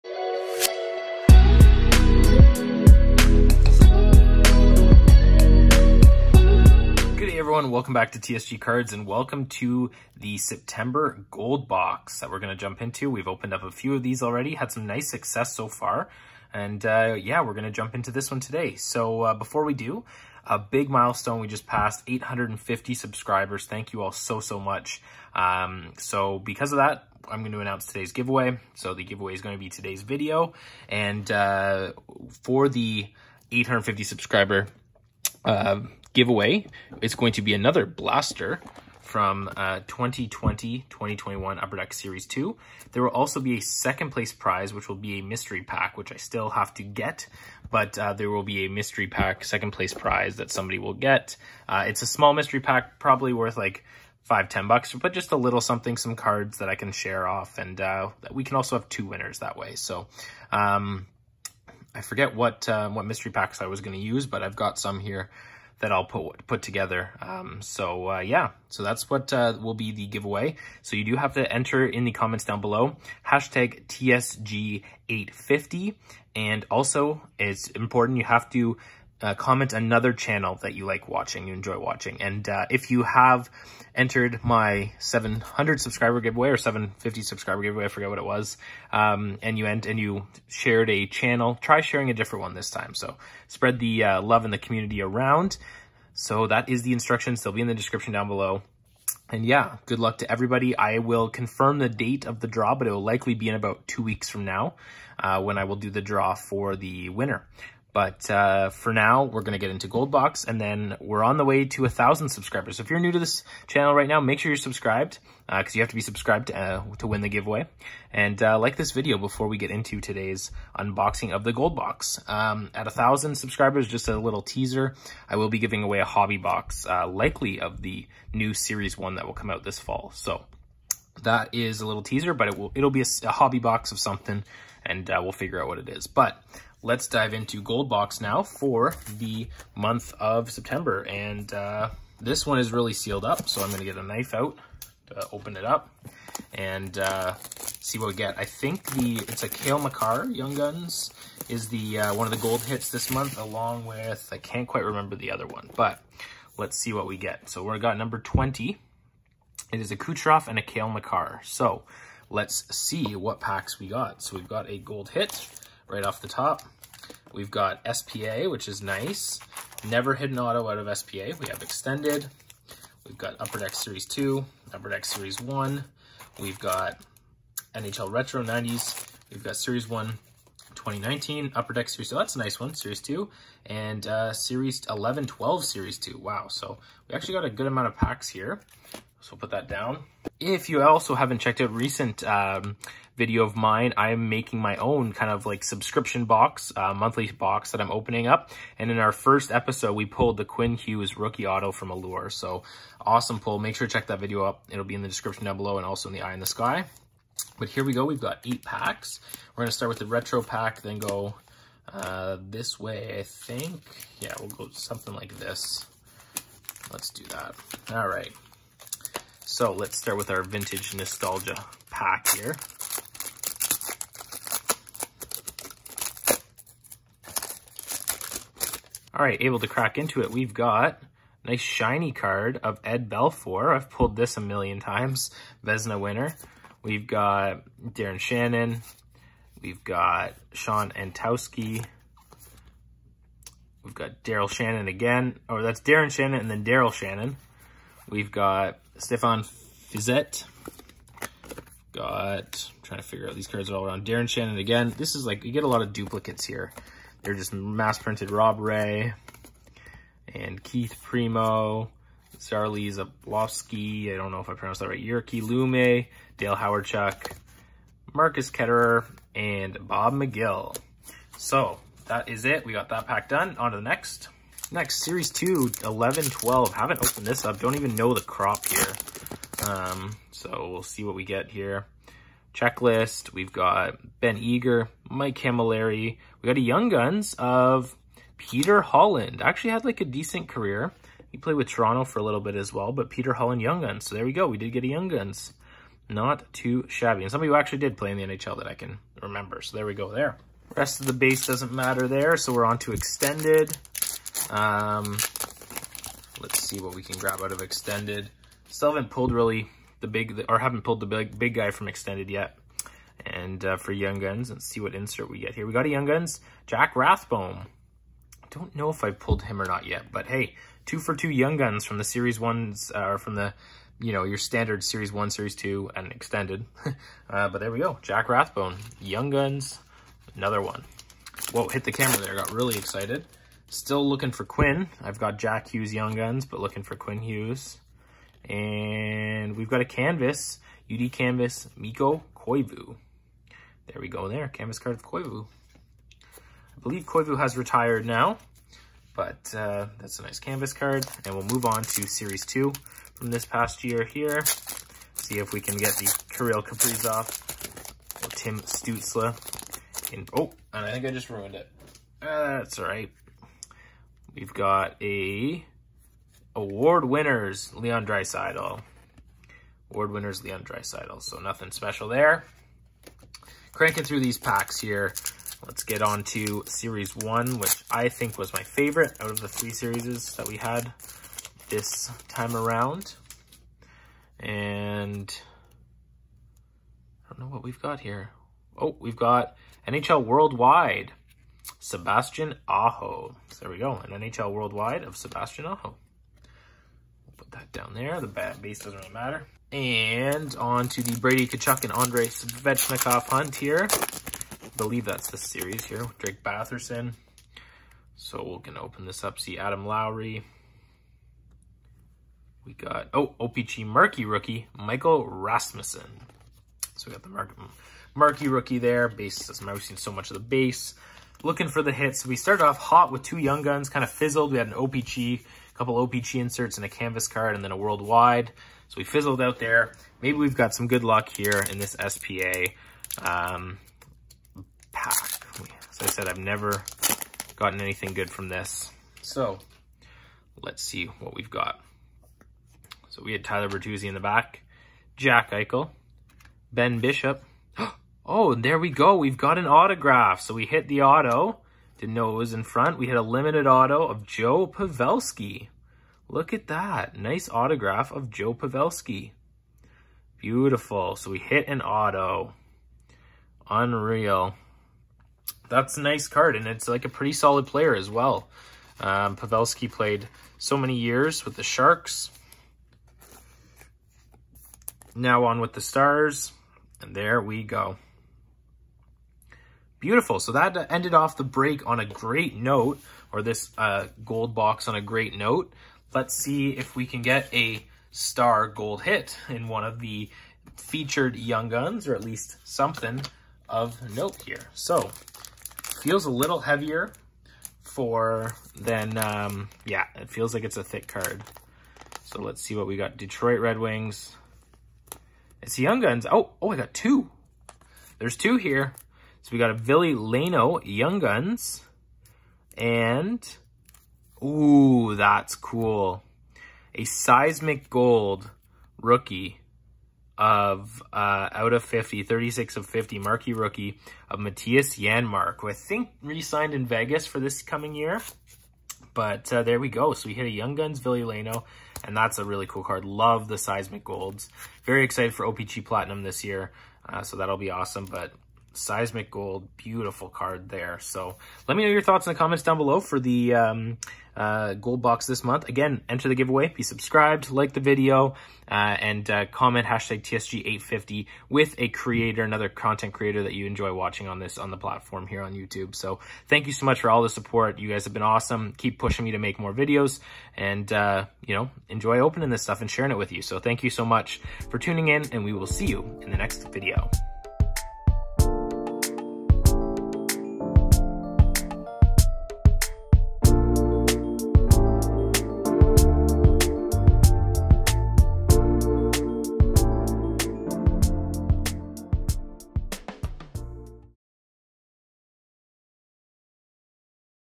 *NEW* GOLDBOX HOCKEY SEPTEMBER! + 850 SUBSCRIBER GIVEAWAY!
A HUGEEEE THANKS to everyone for getting me to 850 subs! Its crazy, lets keep it going! So much appreciated.

1st prize: Blaster (20-21 S2)
2nd prize: Mystery Pack

To Enter:
2. Like this video
3. Comment TSG850 and mention a channel you like watching in the community (must do this to win)